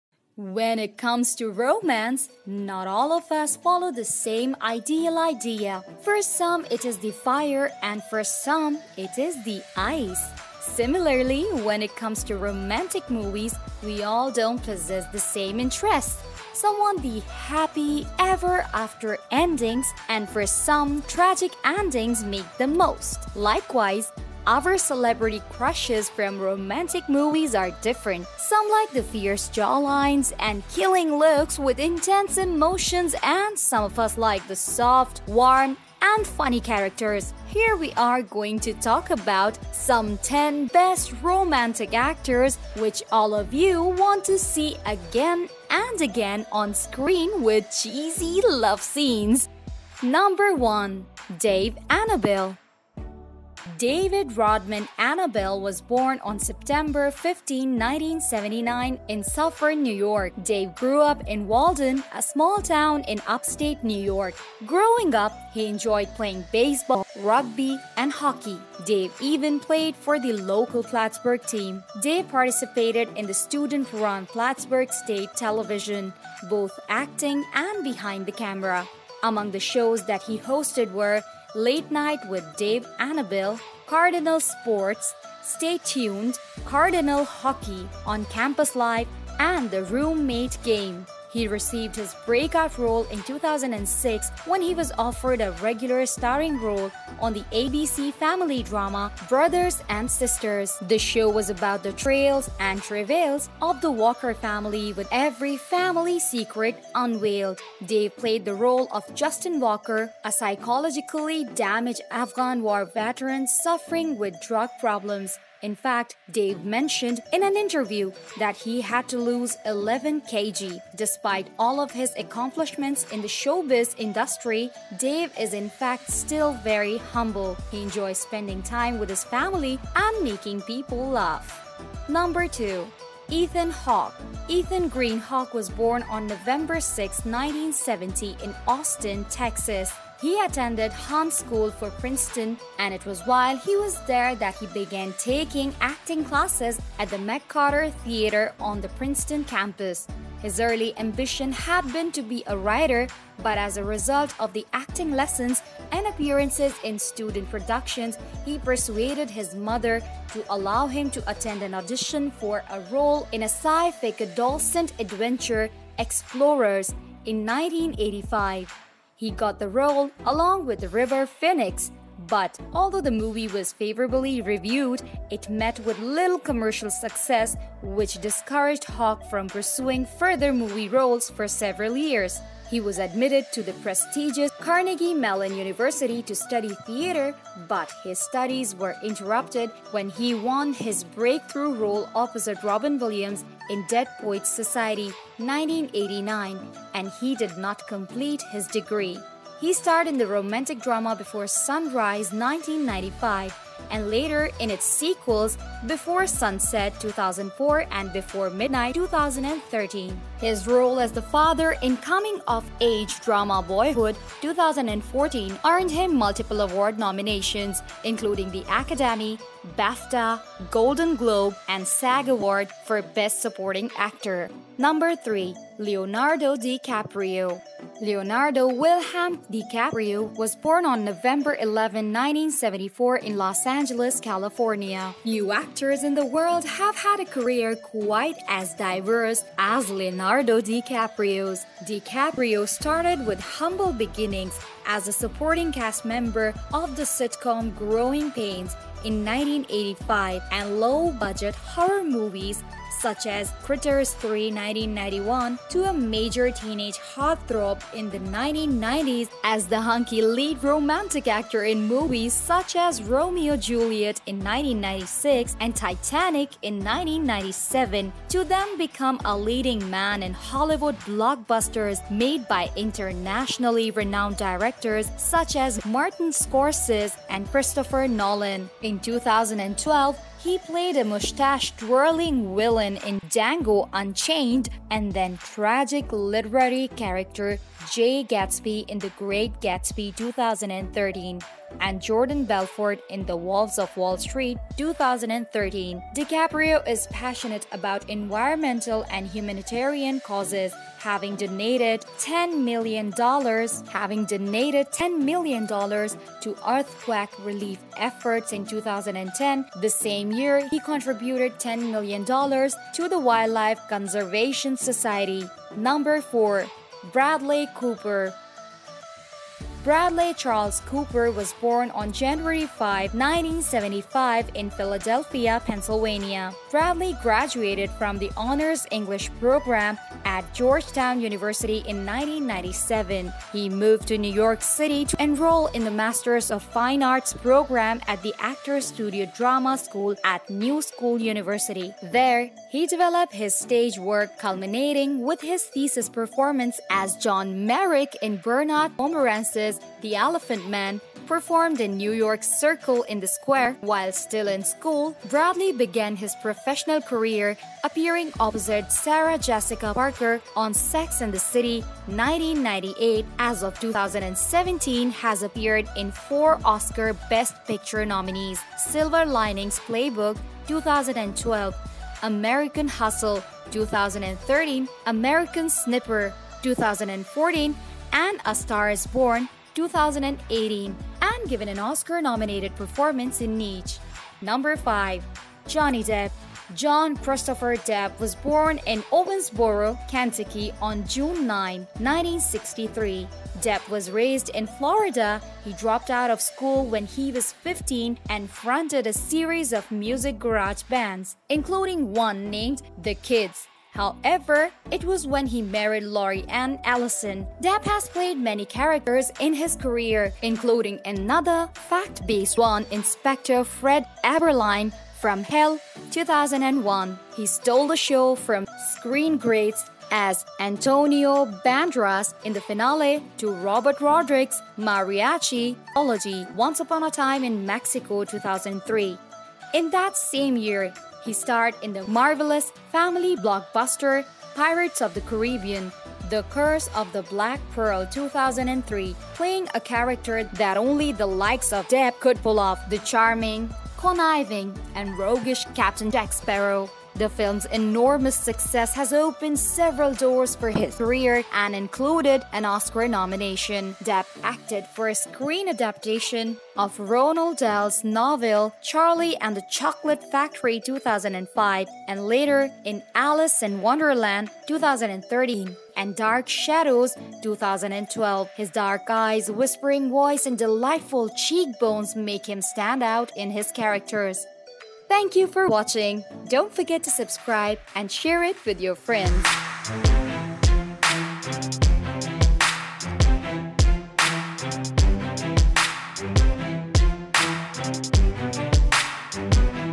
Top 10 Romantic Actors In Hollywood
When it comes to romance not all of us follow the same ideal idea for some it is the fire and for some, it is the ice. Similarly, when it comes to romantic movies we all don’t possess the same interests. Some want the happy ever after endings and for some tragic endings make the most. Likewise, our celebrity crushes from romantic movies are different, some like the fierce jawlines and killing looks with intense emotions, and some of us like soft, warm, and funny characters. Here we are going to talk about the top 5 romantic actors which all of you want to see again and again on screen with cheesy love scenes.

1. David Rodman Annable was born on September 15, 1979, in Suffern, New York. Dave grew up in Walden, a small town in upstate New York. Growing up, he enjoyed playing baseball, rugby, and hockey. Dave even played for the local Plattsburgh team. Dave participated in the student-run Plattsburgh State Television, both acting and behind the camera. He received his breakout role in 2006 when he was offered a regular starring role on the ABC family drama, Brothers & Sisters (2006). The show was about the trials and travails of the Walker family, with every family secret unveiled.

2. Ethan Green Hawke was born on November 6, 1970, in Austin, Texas. He attended Hun School of Princeton and it was while he was there that he began taking acting classes at the McCarter Theatre on the Princeton campus. His early ambition had been to be a writer, but as a result of the acting lessons and appearances in student productions, he persuaded his mother to allow him to attend an audition for a role in a sci-fi adolescent adventure, Explorers (1985). He got the role (along with River Phoenix) but although the movie was favorably reviewed, it met with little commercial success which discouraged Hawke from pursuing further movie roles for several years. He was admitted to the prestigious Carnegie-Mellon University to study theatre but his studies were interrupted when he won his break-through role opposite Robin Williams in Dead Poets Society (1989) and he did not complete his degree.

3. Leonardo Wilhelm DiCaprio was born on November 11, 1974, in Los Angeles, California. Few actors in the world have had a career quite as diverse as Leonardo DiCaprio's. DiCaprio started with humble beginnings, as a supporting cast member of the sitcom Growing Pains (1985) and low budget horror movies, such as Critters 3 (1991), to a major teenage heartthrob in the 1990s, as the hunky lead romantic actor in movies such as Romeo + Juliet (1996) and Titanic (1997), to then become a leading man in Hollywood blockbusters, made by internationally renowned directors such as Martin Scorsese and Christopher Nolan. In 2012, he played a mustache-twirling villain in Django Unchained (2012), and then tragic literary character Jay Gatsby in The Great Gatsby (2013) and Jordan Belfort in The Wolf of Wall Street (2013).

4. Bradley Charles Cooper was born on January 5, 1975, in Philadelphia, Pennsylvania. Bradley graduated from the Honors English program at Georgetown University in 1997, he moved to New York City to enroll in the Masters of Fine Arts program at the Actors Studio Drama School at New School University. There, he developed his stage work, culminating with his thesis performance as John Merrick in Bernard Pomerance's "The Elephant Man", performed in New York's Circle in the Square. While still in school, Bradley began his professional career, appearing opposite Sarah Jessica Parker on Sex and the City (1998).

5. John Christopher Depp was born in Owensboro, Kentucky, on June 9, 1963. Depp was raised in Florida. He dropped out of school when he was 15, and fronted a series of music-garage bands, including one named 'The Kids'. However, it was when he married Lori Anne Allison. Depp has played many characters in his career, including another fact-based one, Insp. Fred Abberline in From Hell (2001). He stole the show from screen greats such as Antonio Banderas in the finale to Robert Rodriguez's "mariachi" trilogy, Once Upon a Time in Mexico (2003). In that same year he starred in the marvelous family blockbuster Pirates of the Caribbean: The Curse of the Black Pearl (2003), playing a character that only the likes of Depp could pull off: the charming, conniving and roguish Capt. Jack Sparrow. The film's enormous success has opened several doors for his career and included an Oscar nomination.